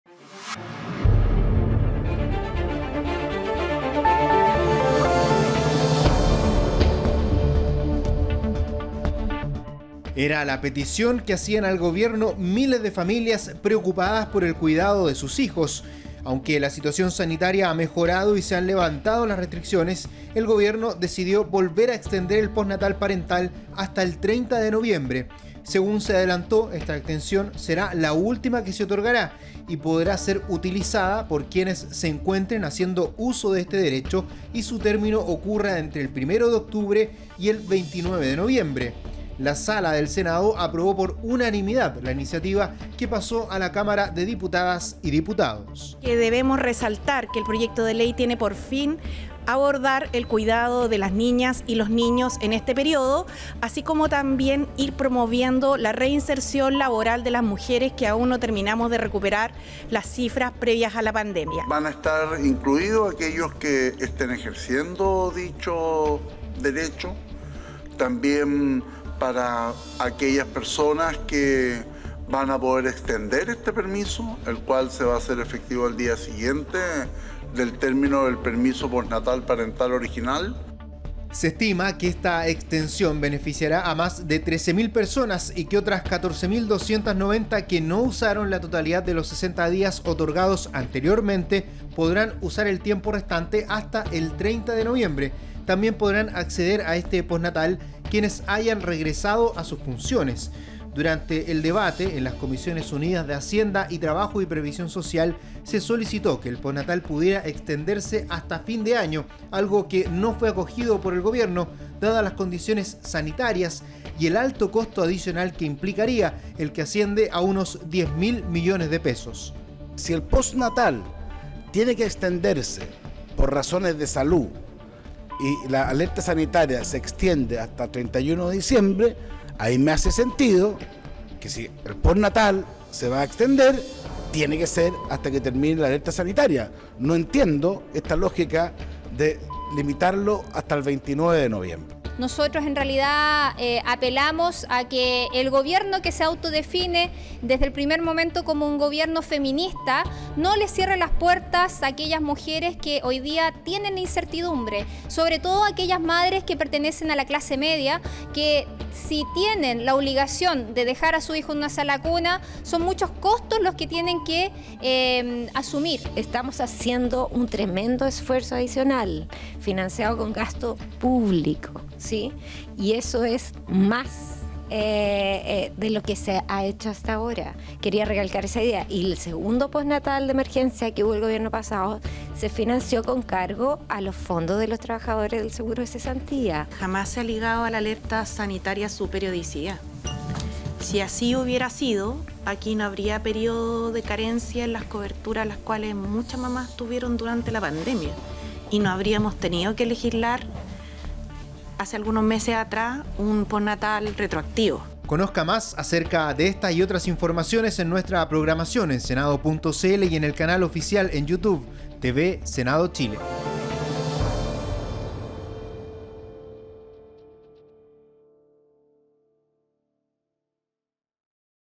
Senado Informa - Senado aprueba extensión del postnatal
Cobertura del beneficio será hasta el 30 de Noviembre

24 de Octubre de 2022